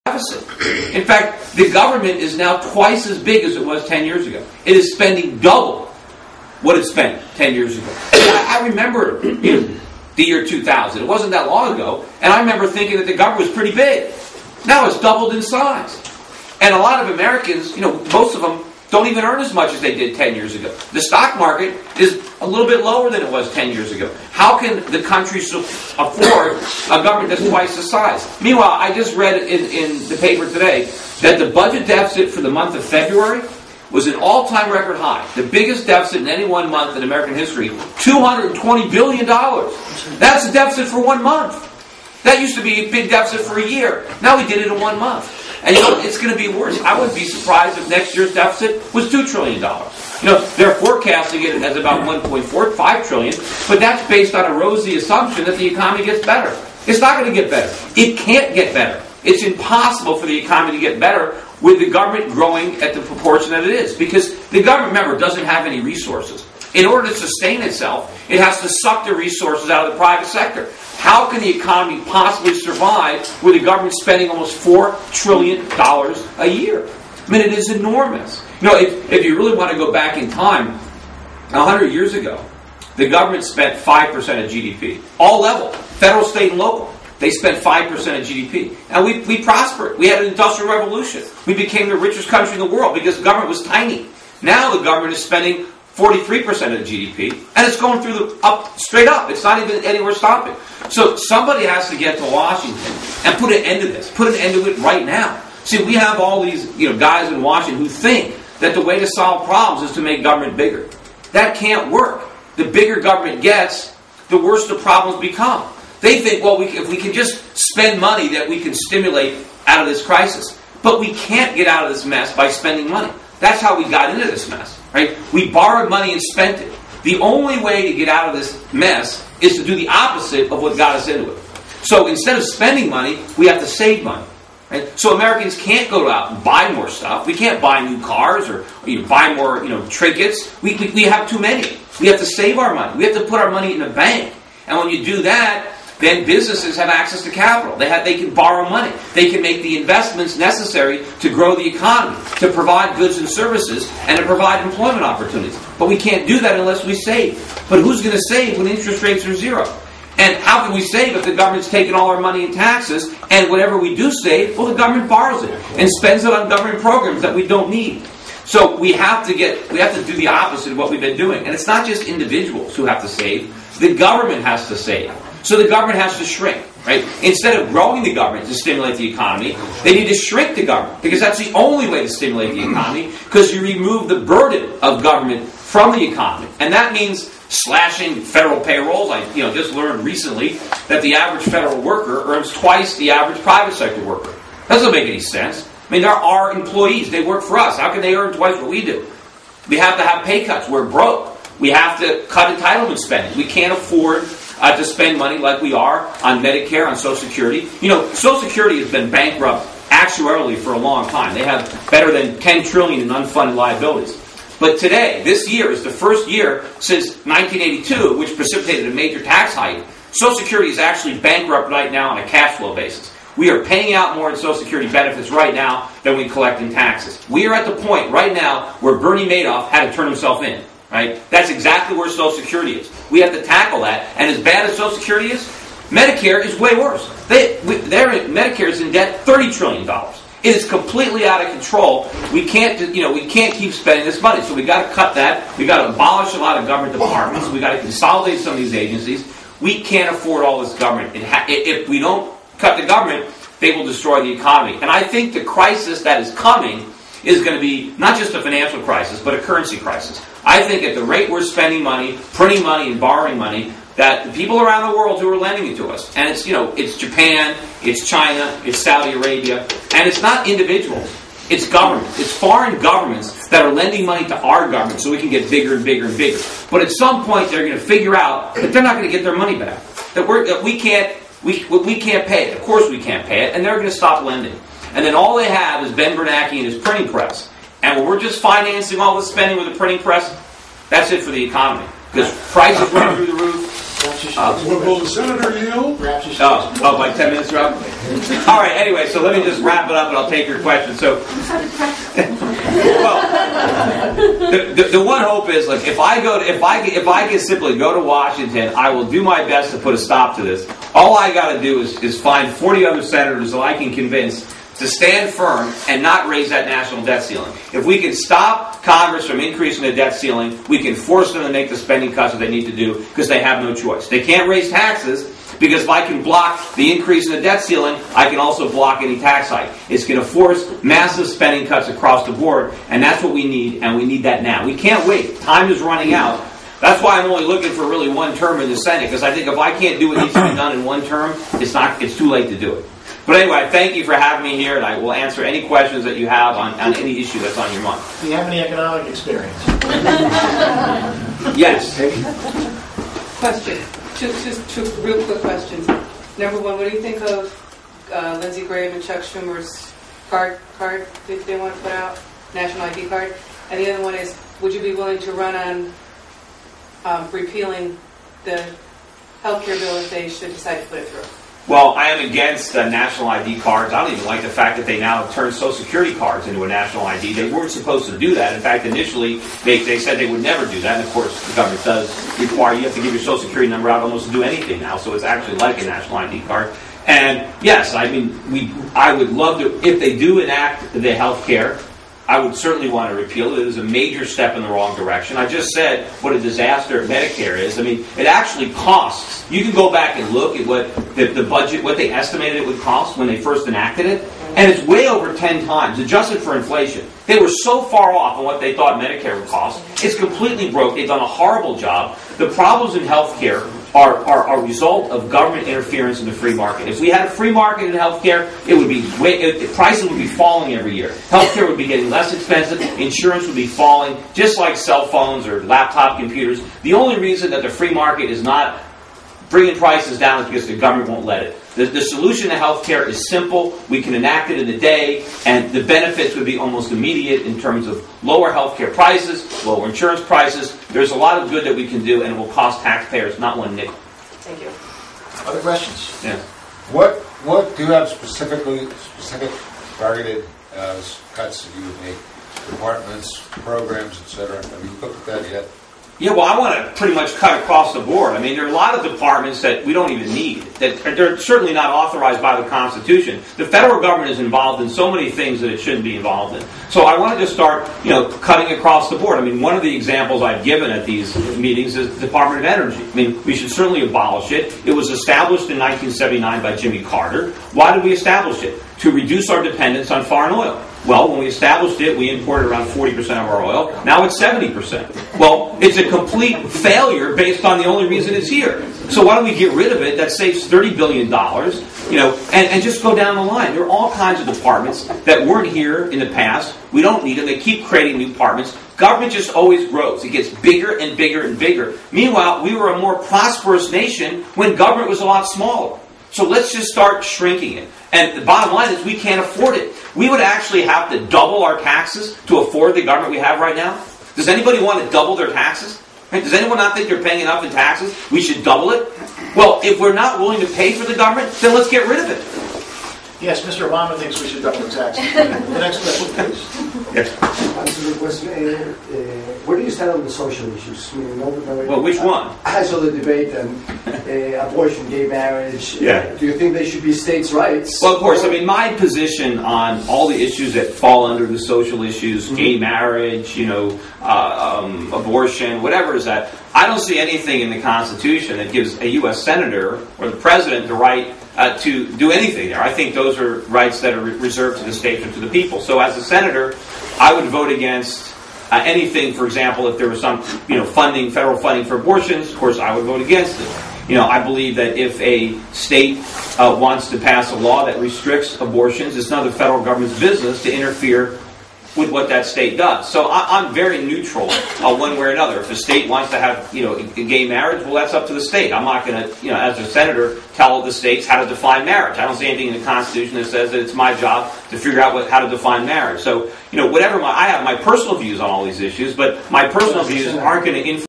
Peter Schiff - Killingworth, CT - P2
Peter Schiff introduces himself to the Killingworth, CT Republican Town Committee on 3-11-10.